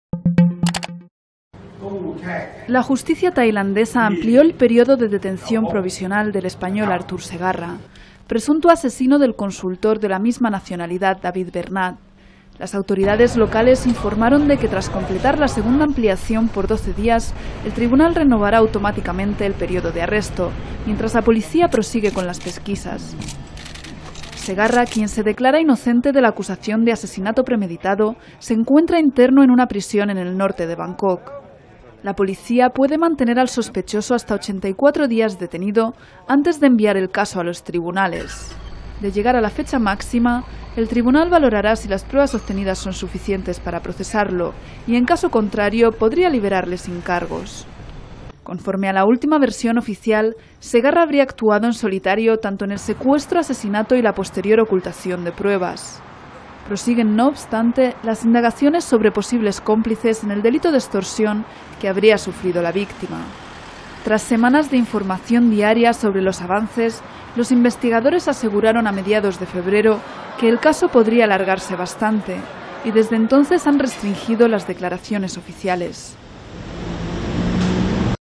La justicia tailandesa amplía la detención provisional del español Artur Segarra
Bangkok, 4 mar (EFE).- La justicia tailandesa amplió hoy el periodo de detención provisional del español Artur Segarra, presunto asesino del consultor de la misma nacionalidad David Bernat, informaron las autoridades locales.
"Tras completar la segunda ampliación por 12 días, el tribunal renovará automáticamente el periodo de arresto mientras la policía prosigue con las pesquisas", comentó a Efe el oficial policial Wasan Wichitputhikul.
Segarra, quien se declara inocente de la acusación de asesinato premeditado entre otros cargos, se encuentra en una interno en la prisión Thantasathan Piset, en el norte de la capital.
La Policía puede mantener al sospechoso hasta 84 días detenido -desde su entrega a las autoridades tailandesas el 8 de febrero-, antes de enviar el caso a los tribunales.
De llegar a la fecha máxima, el tribunal valorará si las pruebas obtenidas son suficientes para procesar al sospechoso y, en caso contrario, podría liberarle sin cargos.
Conforme a la última versión oficial, Segarra, de 37 años, habría actuado en solitario tanto en el secuestro, asesinato y la posterior ocultación de pruebas.
Prosiguen, no obstante, las indagaciones sobre posibles cómplices en el delito de extorsión que habría sufrido la víctima.
Tras semanas de información diaria sobre los avances en el caso, los investigadores aseguraron a mediados de febrero que el caso podría alargarse "bastante" y desde entonces han restringido las declaraciones oficiales.
Segarra fue detenido el 7 de febrero en la ciudad camboyana de Sihanoukville, adonde había huido dos días antes, y fue entregado un día después a las autoridades tailandesas, ante las que admitió conocer a la víctima.
Bernat fue visto por última vez con vida el pasado 20 de enero, un día después de llegar a Tailandia procedente de Irán, en compañía de otro occidental.
Díez días más tarde se encontraron las primeras partes del cadáver de Bernat -de unos 40 años y natural de la provincia catalana de Lérida-, en el río Chao Phraya, que atraviesa Bangkok y donde luego fueron hallados otros trozos del cuerpo diseminados en el agua.
El equipo forense determinó que la víctima falleció por asfixia entre el 25 y el 27 de enero.
La policía tailandesa cree que el motivo del secuestro y extorsión de Bernat tuvo como objetivo lograr el acceso a sus cuentas bancarias en Singapur, de donde se transfirieron "grandes cantidades de dinero" a la cuenta de Segarra.